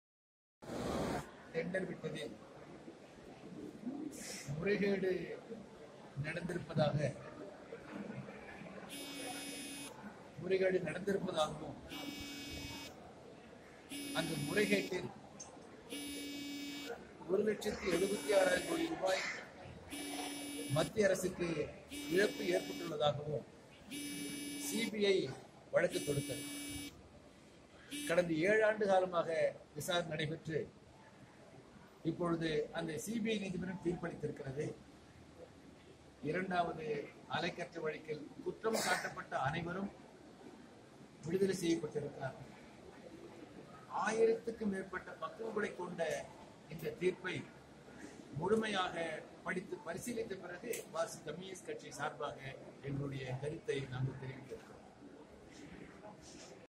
2GScamVerdict குறித்து CPIM மாநிலச் செயலாளர் தோழர் ஜி.ராமகிருஷ்ணன்
2G அலைக்கற்றை ஒதுக்கீட்டில் முறைகேடு நடைபெற்றதாக சிபிஐ சிறப்பு நீதிமன்றத்தில் நடைபெற்று வந்த வழக்கில் இன்று தீர்ப்பு வெளியாகியுள்ளது. இவ்வழக்கில் குற்றம் சாட்டப்பட்ட அனைவரும் நீதிமன்றத்தால் விடுவிக்கப்பட்டுள்ளனர். தீர்ப்பு விபரங்களை முழுமையாக வாசித்த பின்னர் மார்க்சிஸ்ட் கட்சி தனது கருத்தை தெரிவிக்கும் - தோழர் ஜி.ராமகிருஷ்ணன், மாநிலச் செயலாளர்.